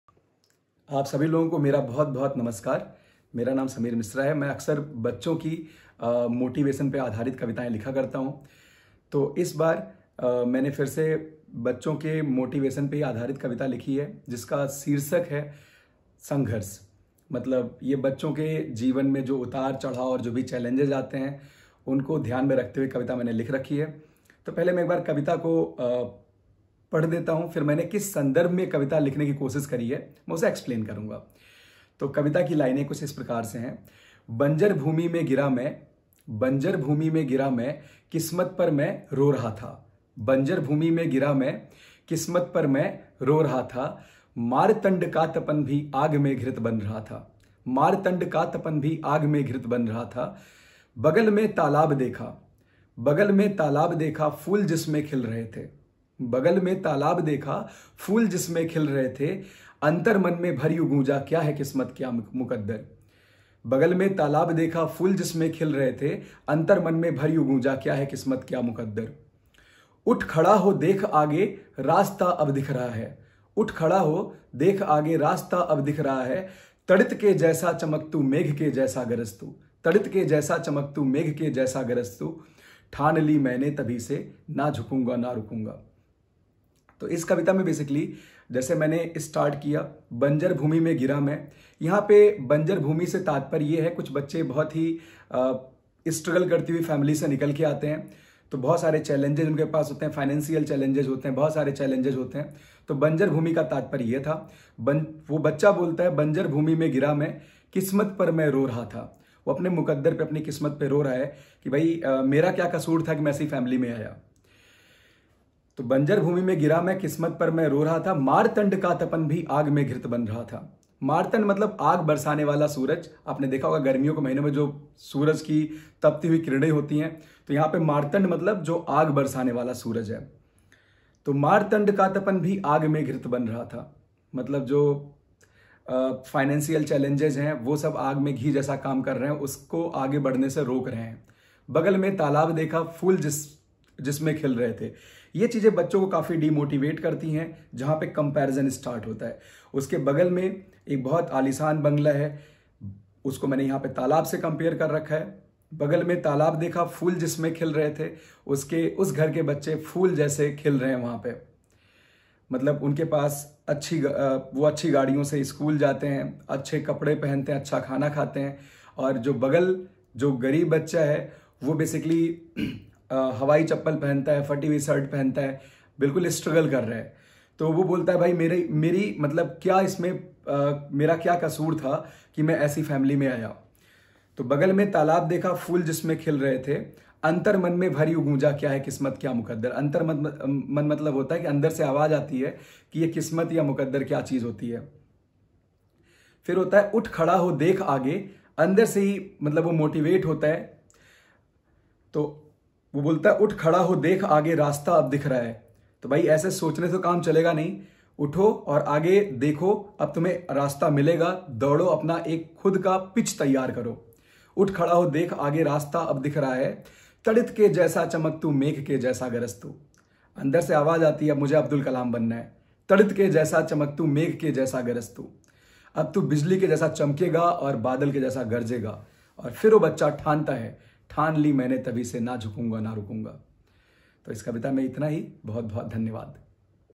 संघर्ष (कविता)
1)संघर्ष कविता उन बच्चों के जीवन पर आधारित है जो बहुत ही विनम्र पृष्ठभूमि (Humble Background) से आते हैं।
2)जीवन में संघर्ष को ताकत कैसे बनाएं।
3)Struggle